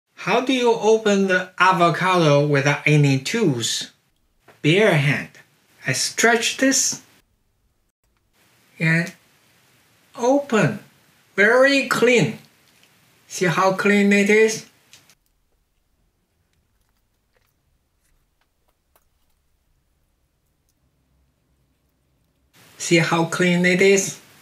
Eat Avocado Cleanly With Bare Hands Without Knife, Spoon or Paper Towel
How do you eat an avocado when you don't have any tools? i.e. you don't have a knife, spoon or any paper towels. Our one requirement is: your hands should be clean after you finish eating it.

I understand that kids should have table manners, but when you are not at home, dining etiquette may not be your top priority. For example when you are camping, it is the time for you to show the kids your fun side: bare-handed avocado eating.

Or, if there are problems, I can reformat it and publish a regular reformat for the viewers.